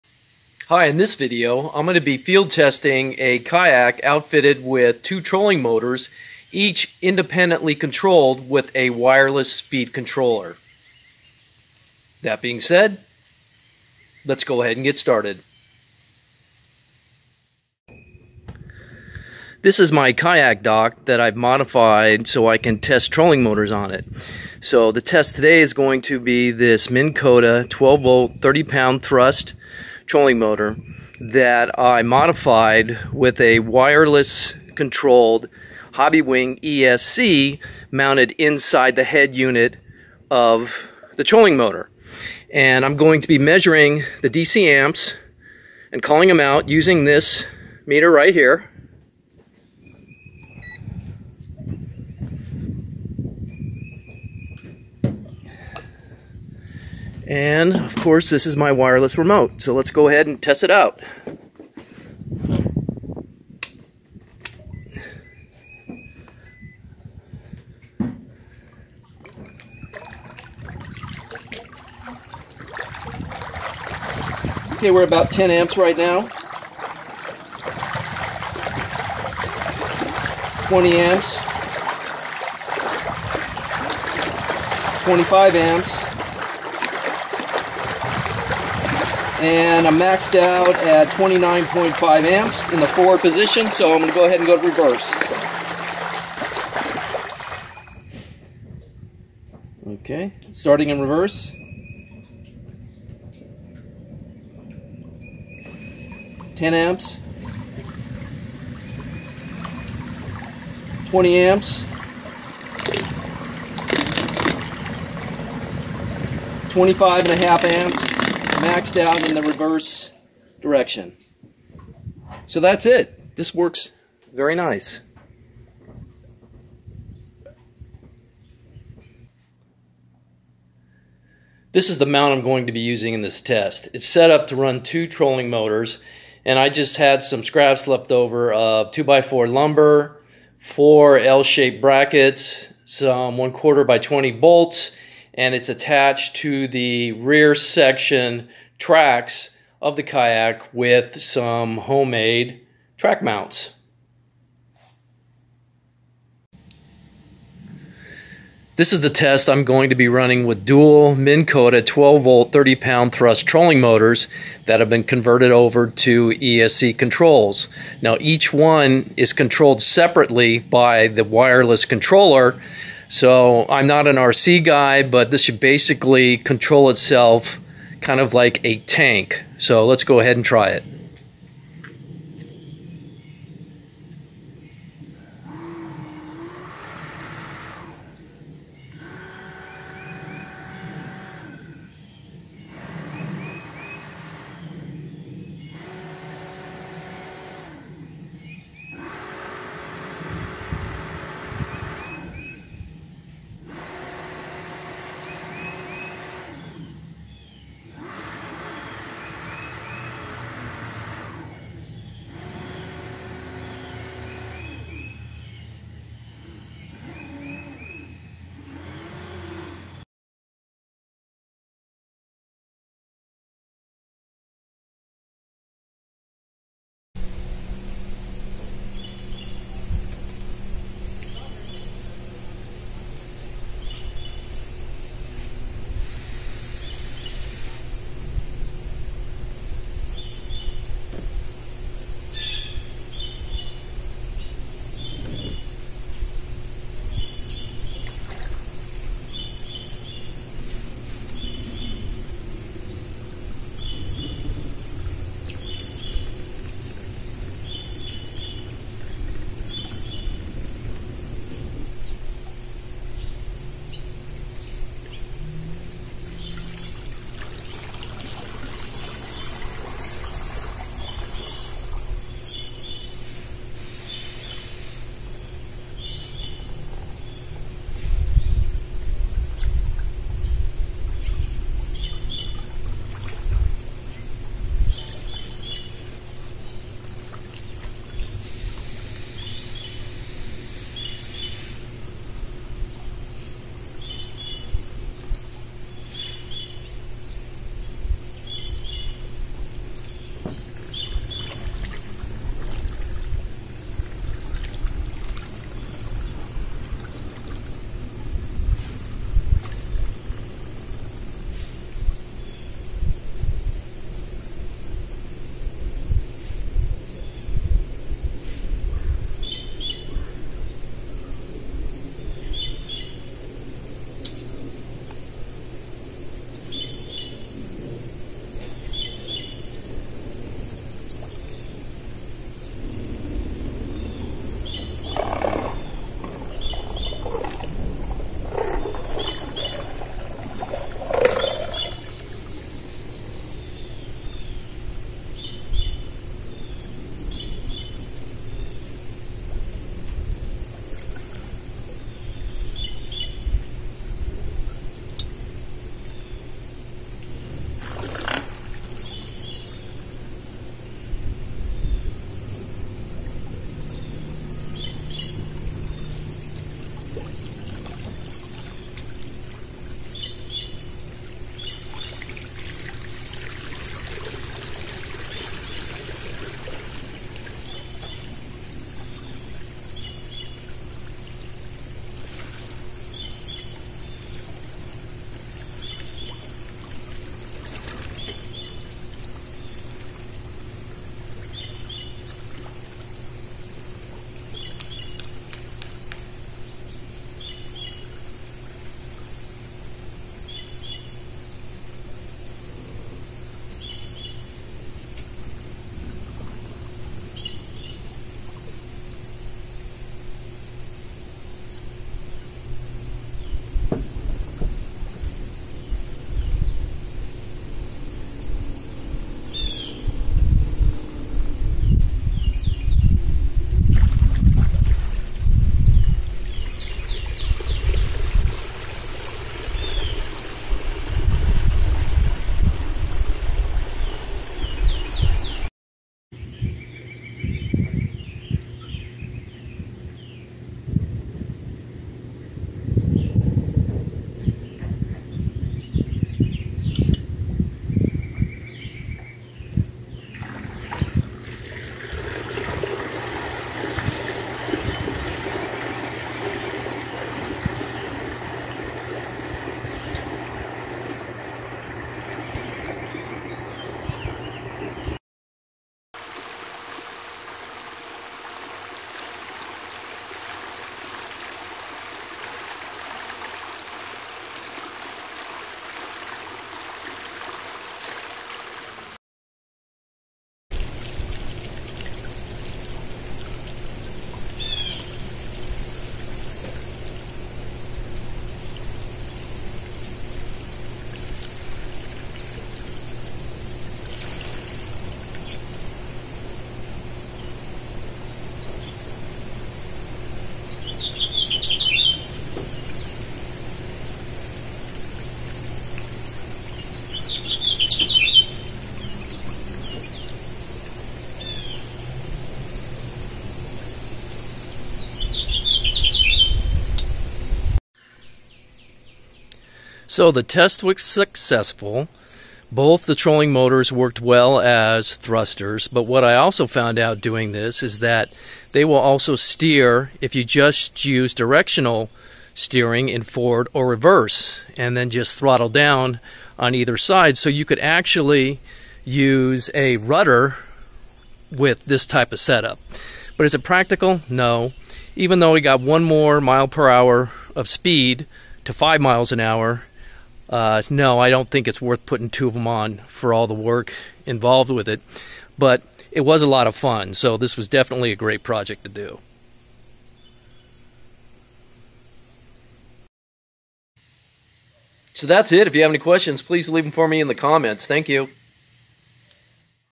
Dual Trolling Motors With Wireless Controllers Used As Thrusters
In this video, I illustrate how I configured dual 12V 30 lb thrust Minn Kota trolling motors with independent wireless controllers as thrusters on a kayak. The wireless controllers acted in a similar manner as an RC tank set up. The trolling motors also performed as directional or differential steering for the kayak. A rudder could be added when used in that configuration to assist in steering. The kayak also reached a speed of 5MPH which was 1MPH greater than 2 previous tests using different brands and sizes of trolling motors.
I used the wireless controlled trolling motors because I had already had built them in previous projects. You could save money and time by using 2 wired servo tester remotes instead. You could also mount the 2 servos into one remote control box for easier use.